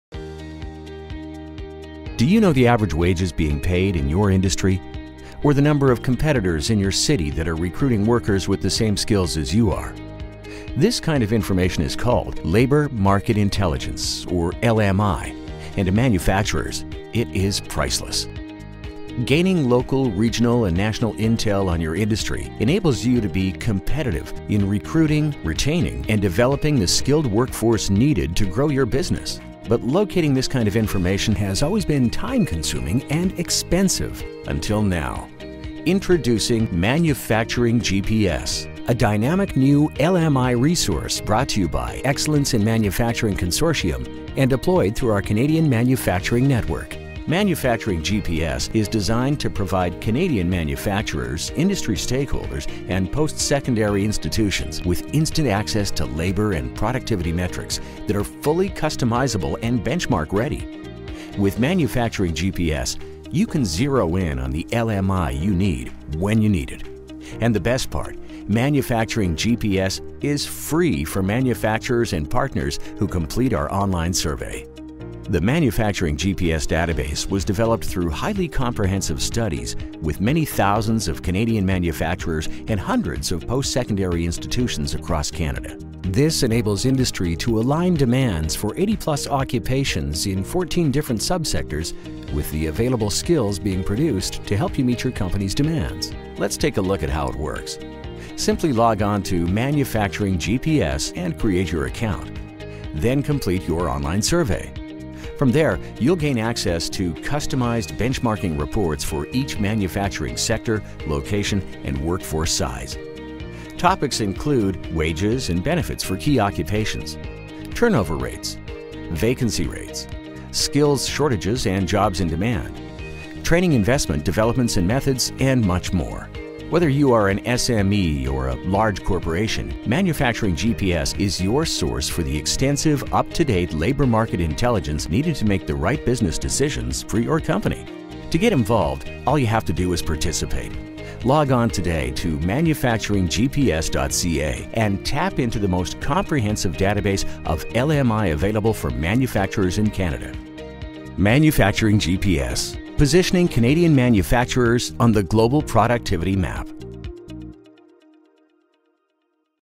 ManufacturingGPS Overview
Developed by Excellence in Manufacturing Consortium (EMC and delivered through our Canadian Manufacturing Network, ManufacturingGPS is a fully searchable Labour Market Intelligence (LMI) system, for Canadian Manufacturers, Industry Stakeholders, Educators, and Community/Government partners. for more details.